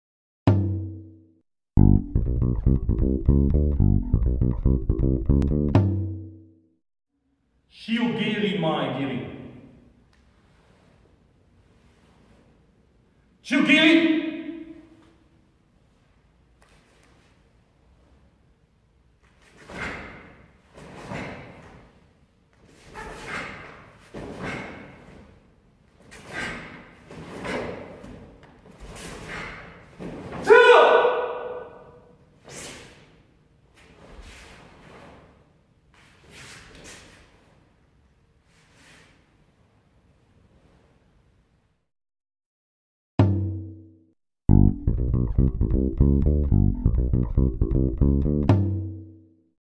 Karate Kata "Shiho Geri - mae geri" - Shito-ryu Shukokai Karate
Die Karate Kata "Shiho Geri - mae geri" für Karate Gürtelprüfungen im Shito-ryu Shukokai Karate in Deutschland - Tom Straphel, Präsident und technischer Leiter des Karateverbandes Shito-ryu Shukokai Union Deutschland e.V. zeigt die Kata "Shiho Geri - mae geri" für alle, die nochmal üben möchten.